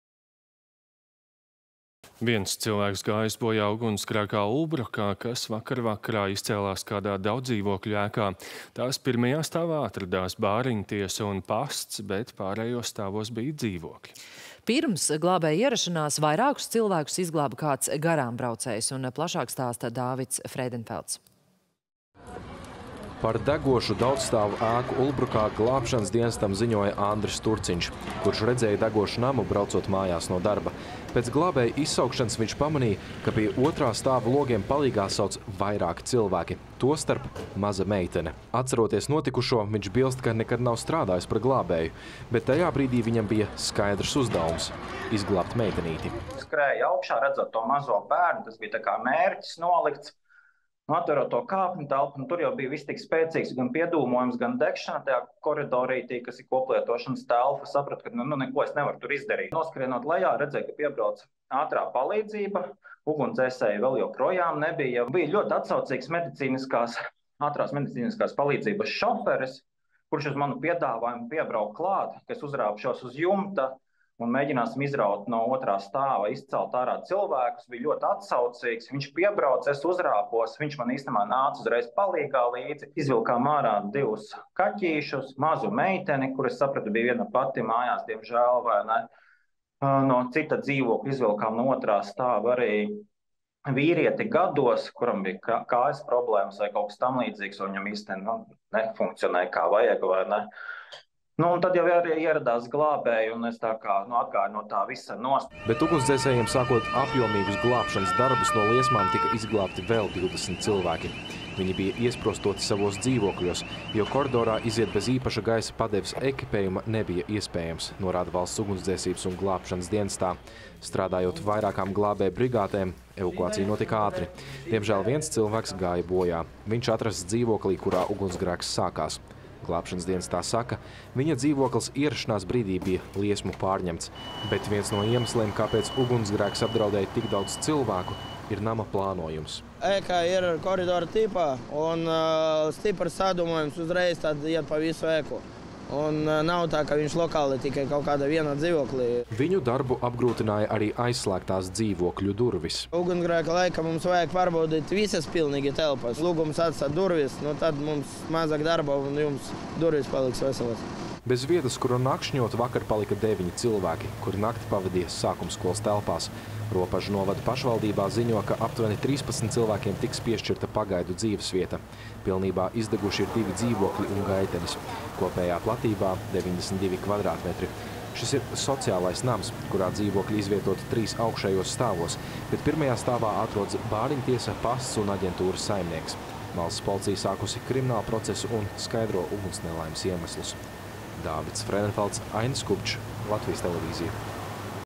Degot daudzdzīvokļu mājai, iet bojā cilvēks
Viens cilvēks gājis bojā ugunsgrēkā Ulbrokā, kas vakar vakarā izcēlās kādā daudzdzīvokļu ēkā. Tās pirmajā stāvā atradās bāriņtiesa un pasts, bet pārējos stāvos bija dzīvokļi. Pirms glābēju ierašanās vairākus cilvēkus izglāba kāds garāmbraucējs. Plašāk stāsta Dāvids Freidenfelds.